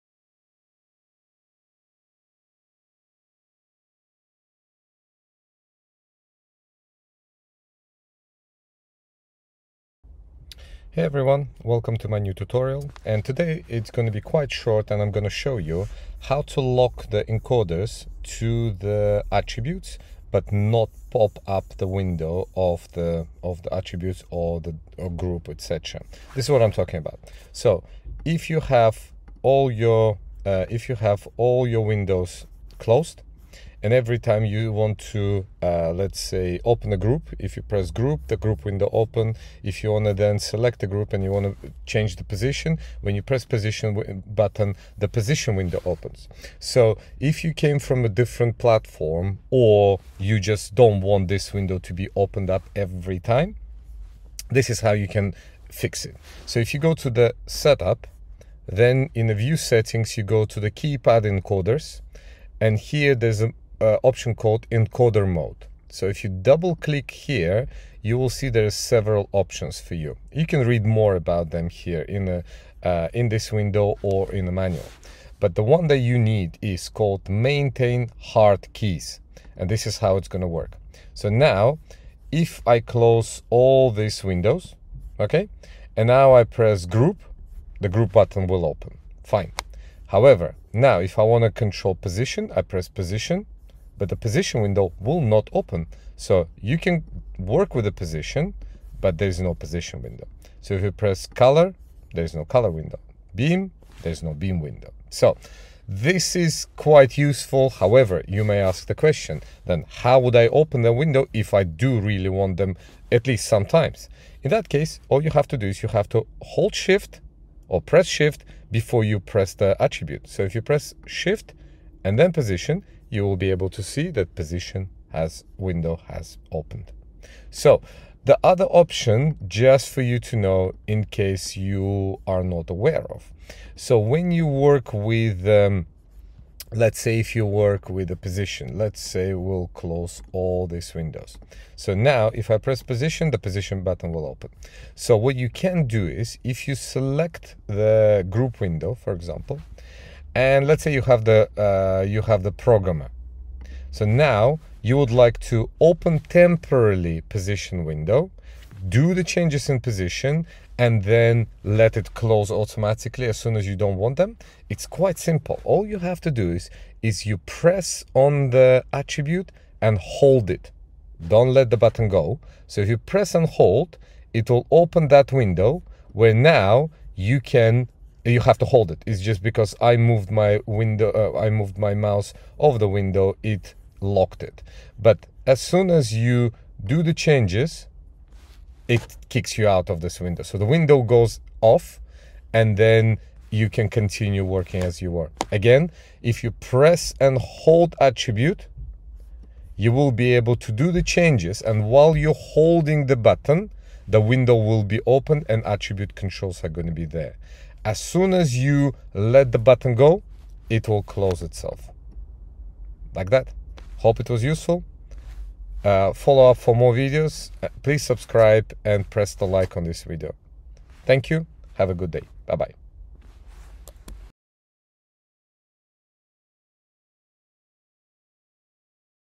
Encoder mode - Maintain Hard Keys
In this tutorial I'll demonstrate how to select attributes but STOP palette windows from being opened.

Setup - View Settings - Encoder/Keypad - Encoder mode - Maintain Hard keys